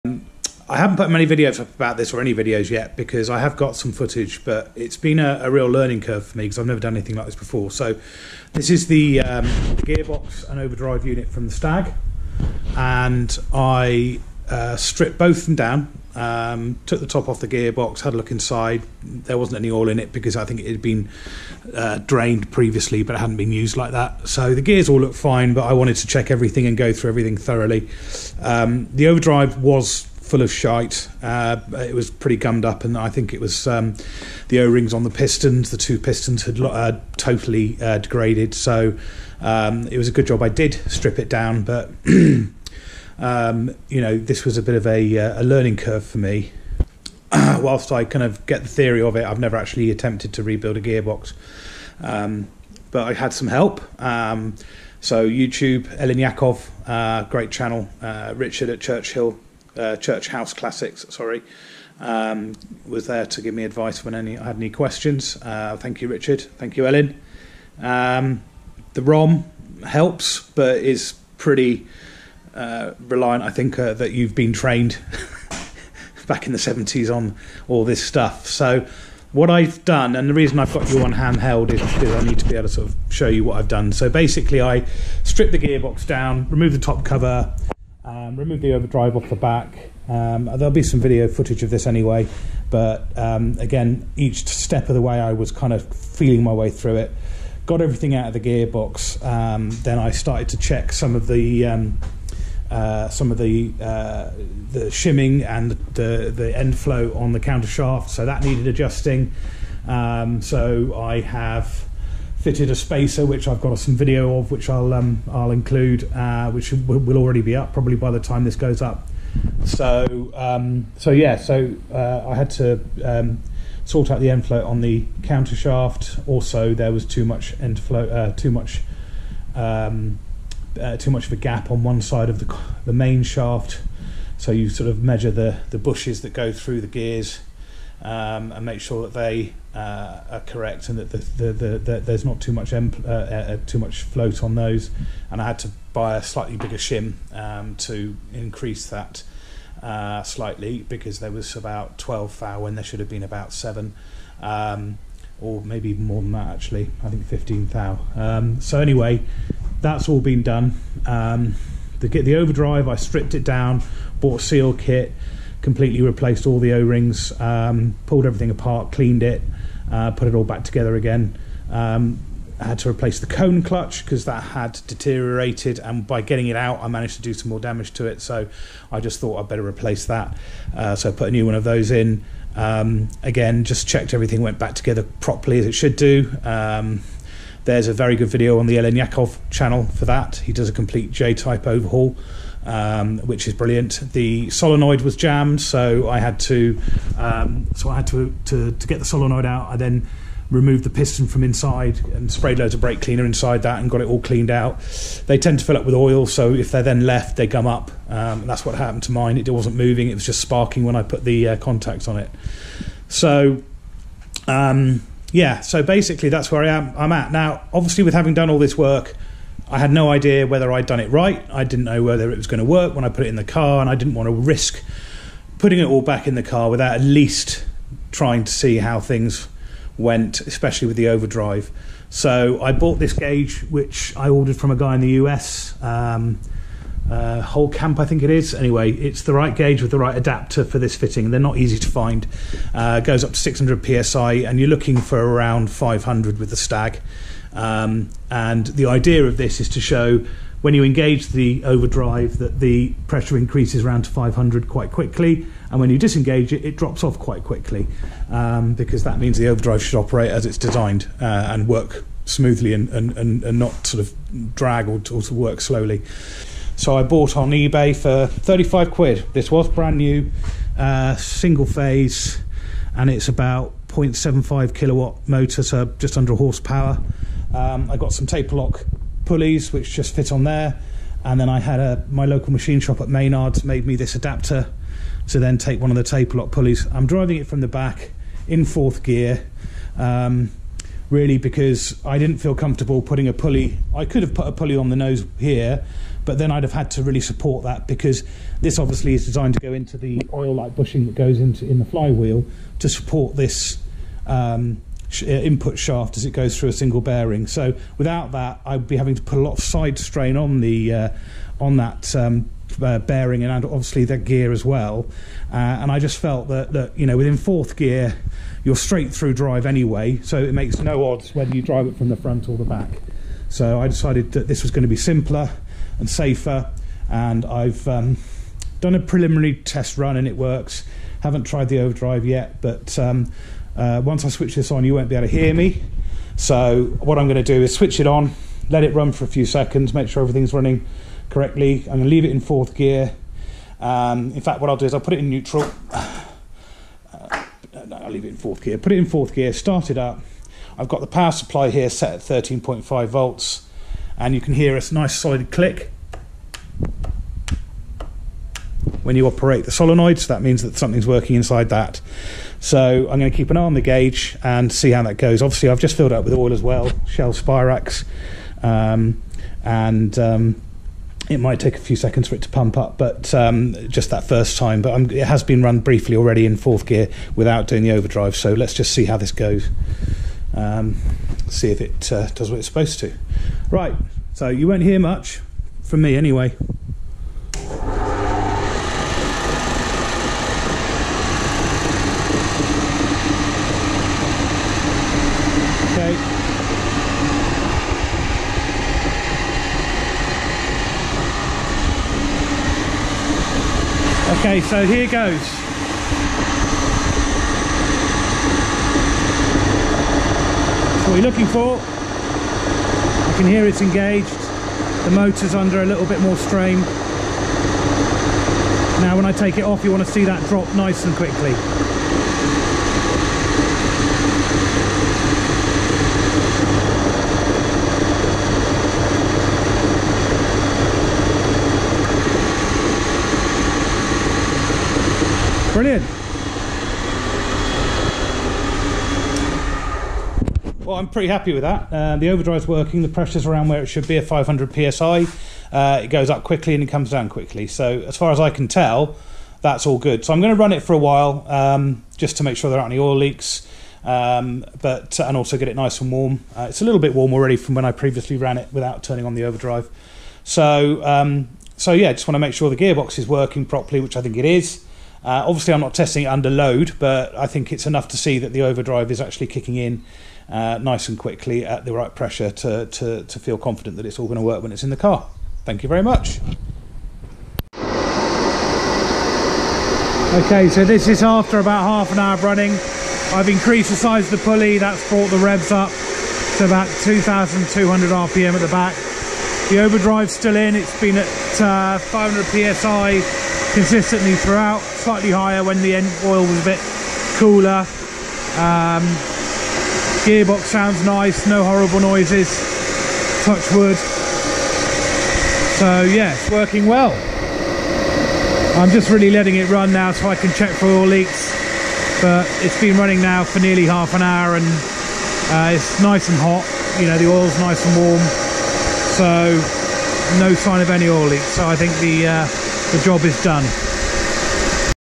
Triumph Stag - Gearbox and Overdrive Testing
Testing Rig for Gearbox from Triumph Stag, also J-Type Overdrive.